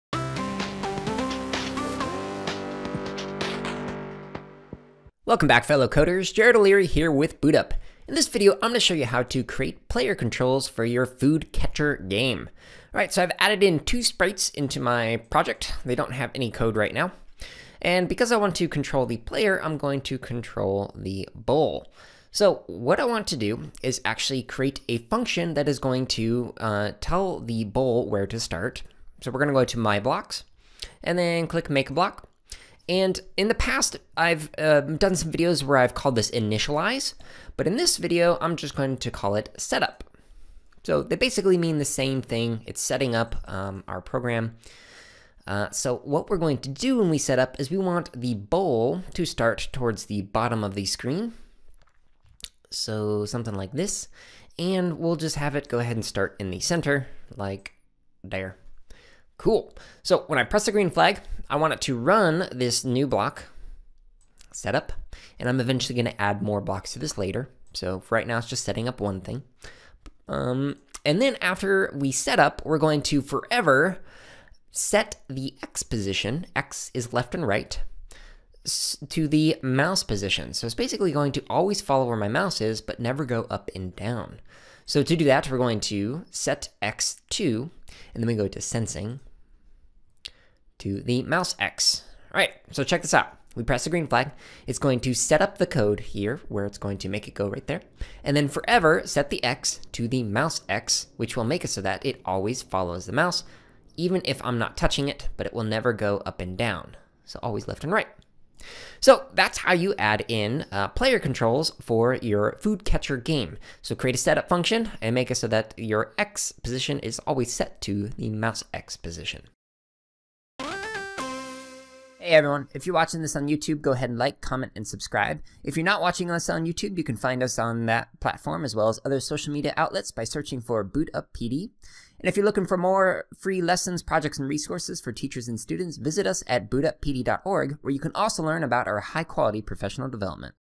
Food Catcher - Create Player Controls
Coders create a food catcher game that keeps track of the player’s score. The purpose of this project is to introduce variables to keep track of a player’s score.